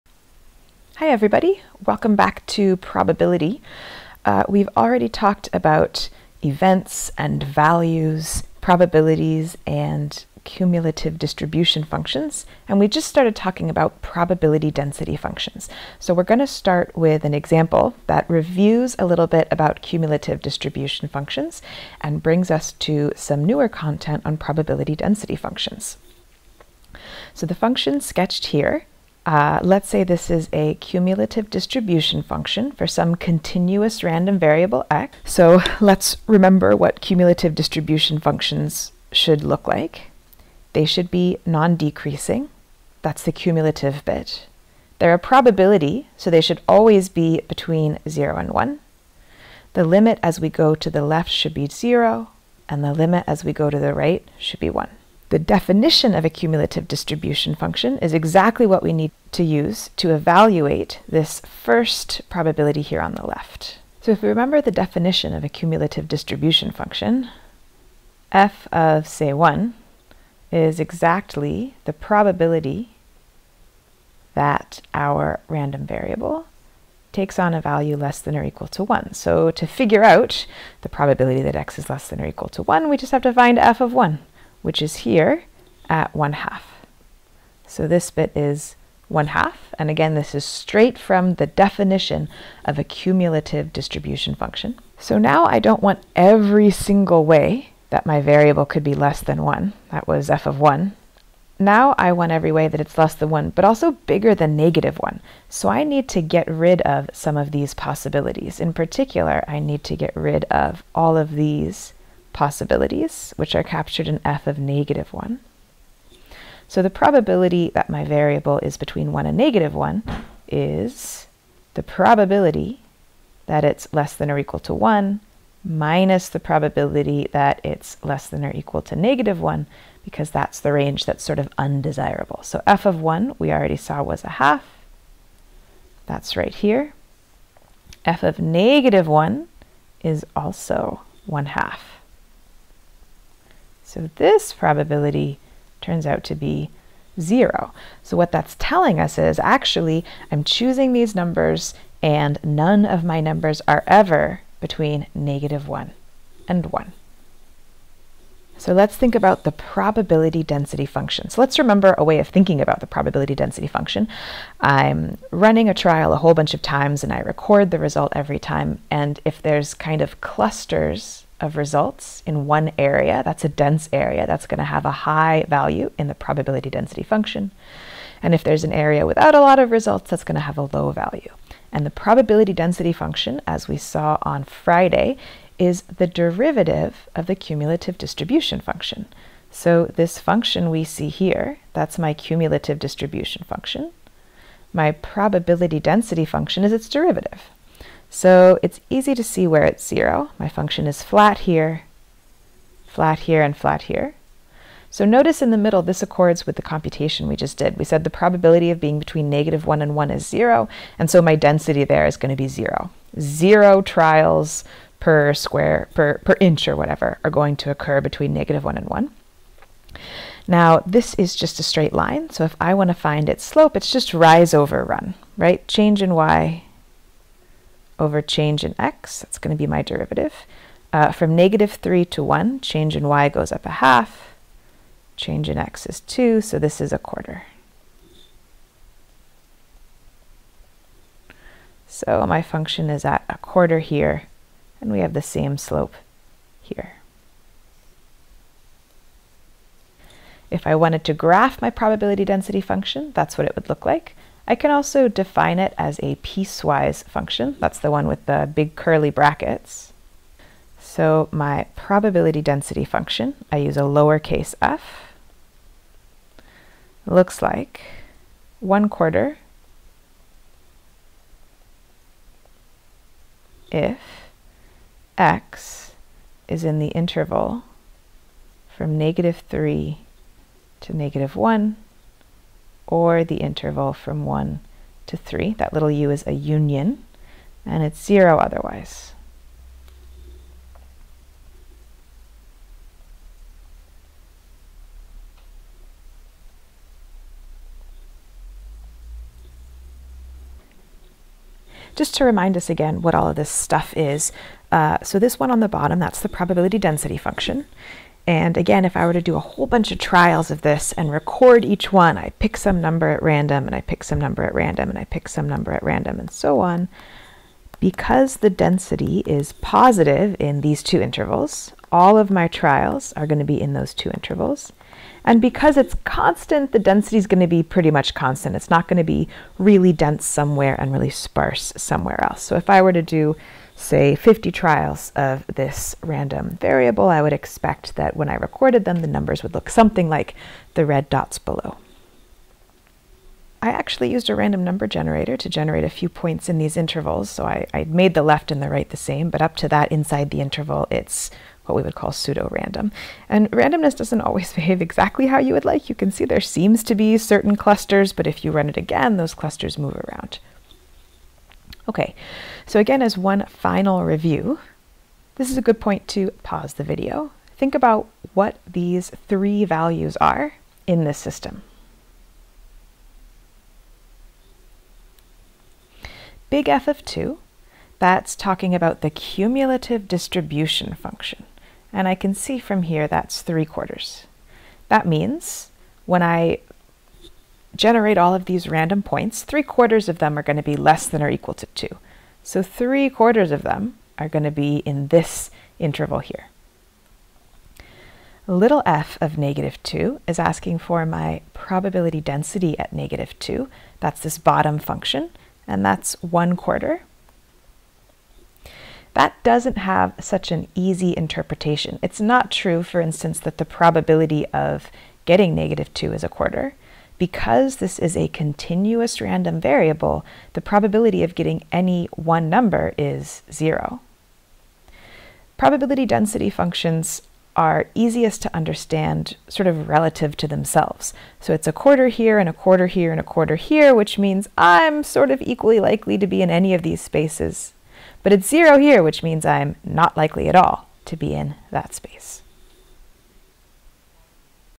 Probability Lecture 10
An example of finding a PDF from a CDF.